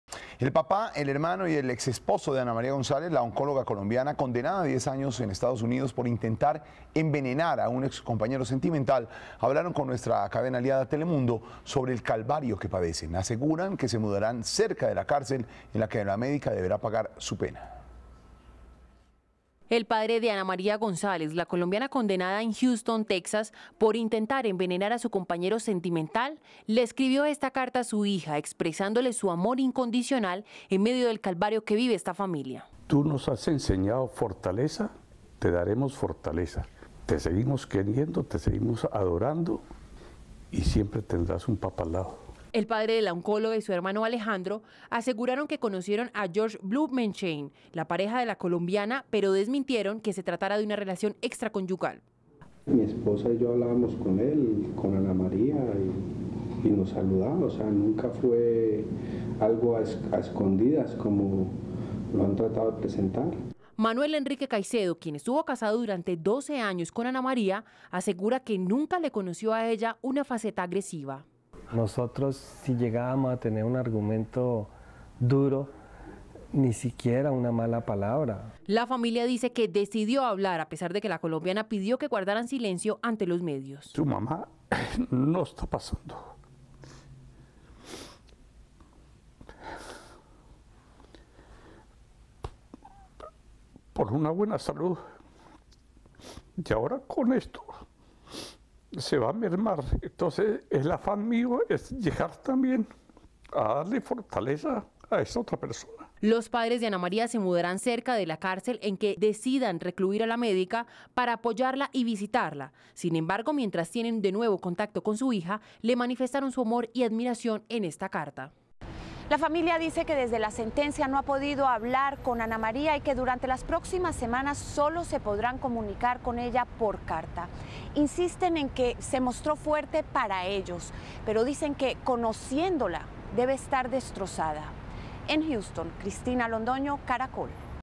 Oncóloga condenada en EE. UU. - 30 de Septiembre de 2014
Él y el padre de la médica colombiana, sentenciada a 10 años de cárcel por supuestamente intentar envenenar a un examante, están seguros de su inocencia.